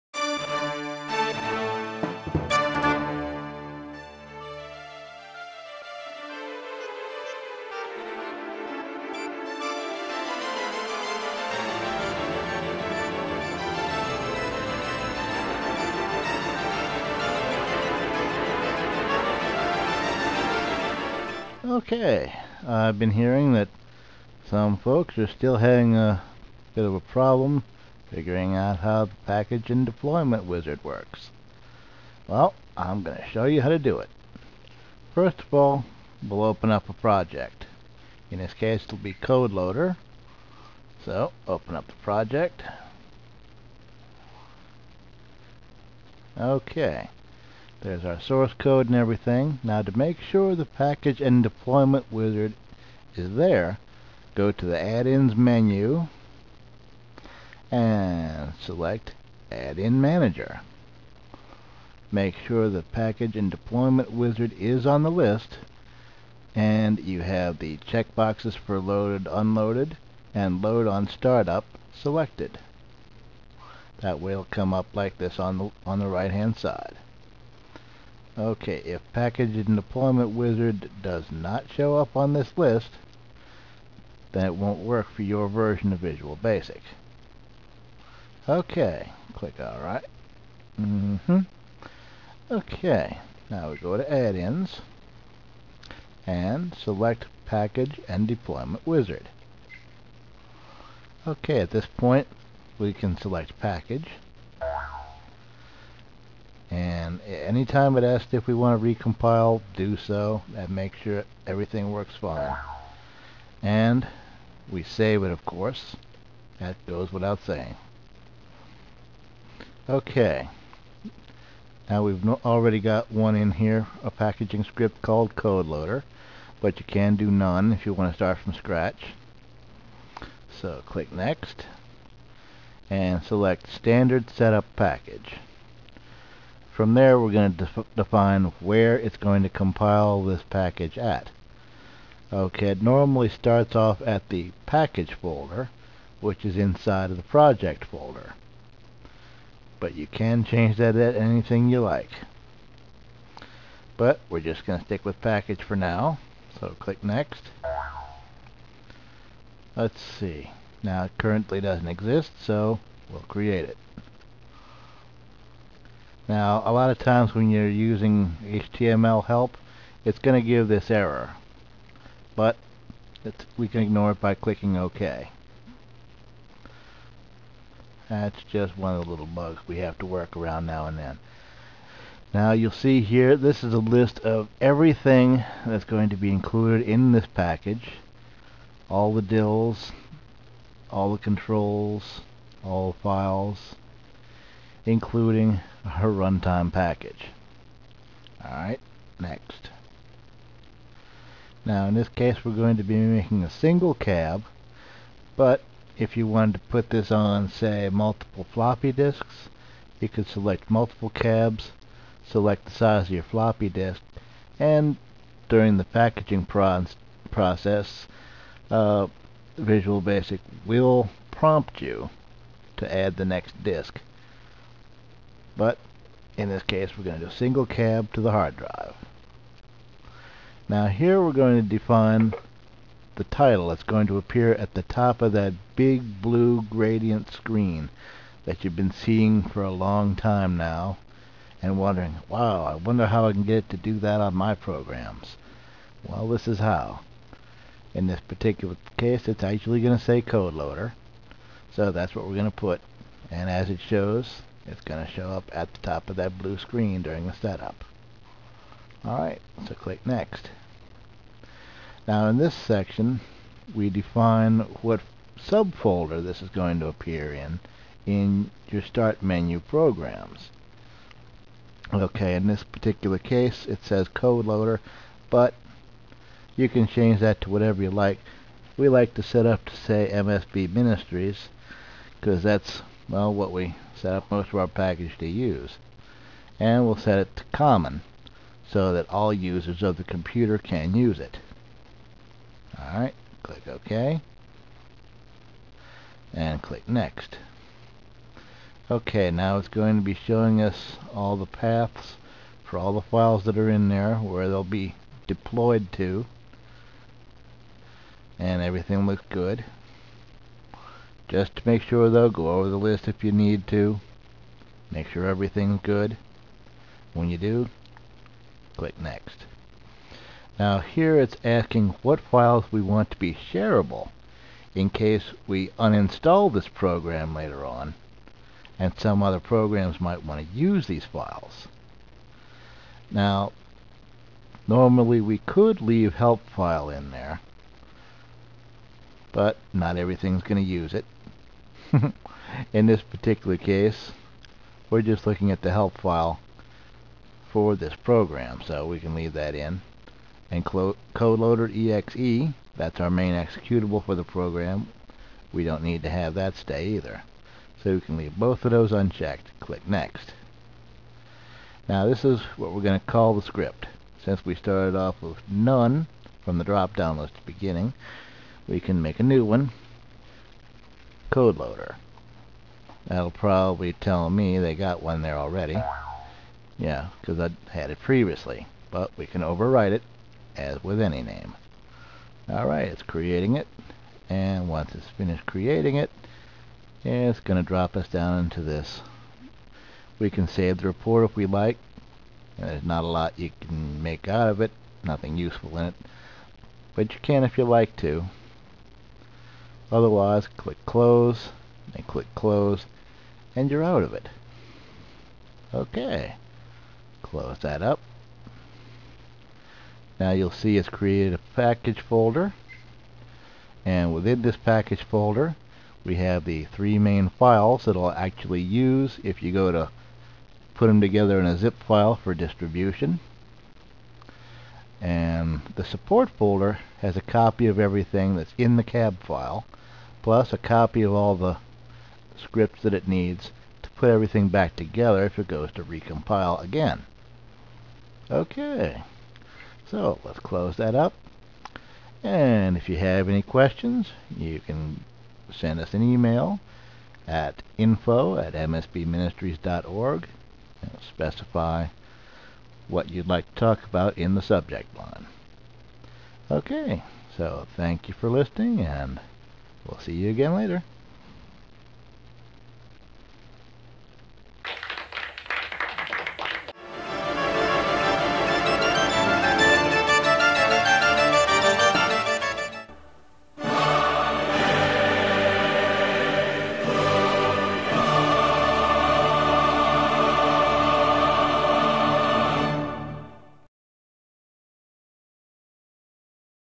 Using The Package And Deployment Wizard
The right way to use the Microsoft Package And Deployment Wizard, to convert your completed Visual Basic project, into an easily distributable setup package.